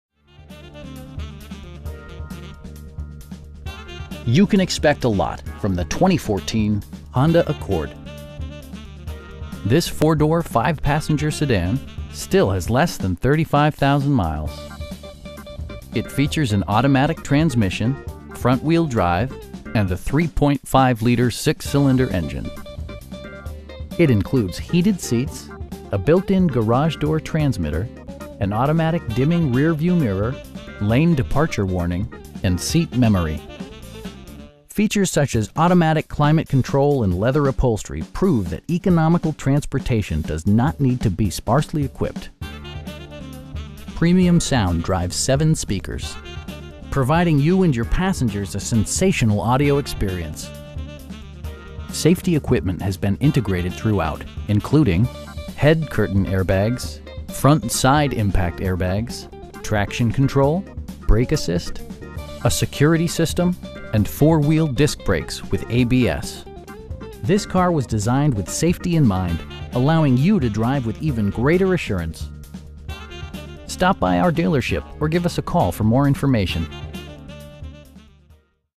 2014 Honda Accord EX-L in Kingwood, TX 77339
Sterling McCall Honda
22575 Highway 59 N

You can expect a lot from the 2014 Honda Accord. This 4 door, 5 passenger sedan still has less than 35,000 miles! It features an automatic transmission, front-wheel drive, and a 3.5 liter 6 cylinder engine. It includes heated seats, 1-touch window functionality, a built-in garage door transmitter, an automatic dimming rear-view mirror, lane departure warning, power windows, an overhead console, and seat memory. Features such as automatic climate control and leather upholstery prove that economical transportation does not need to be sparsely equipped. For drivers who enjoy the natural environment, a power moon roof allows an infusion of fresh air. Premium sound drives 7 speakers, providing you and your passengers a sensational audio experience. Safety equipment has been integrated throughout, including: head curtain airbags, front side impact airbags, traction control, brake assist, a security system, and 4 wheel disc brakes with ABS. This car was designed with safety in mind, allowing you to drive with even greater assurance. We pride ourselves in the quality that we offer on all of our vehicles. Stop by our dealership or give us a call for more information.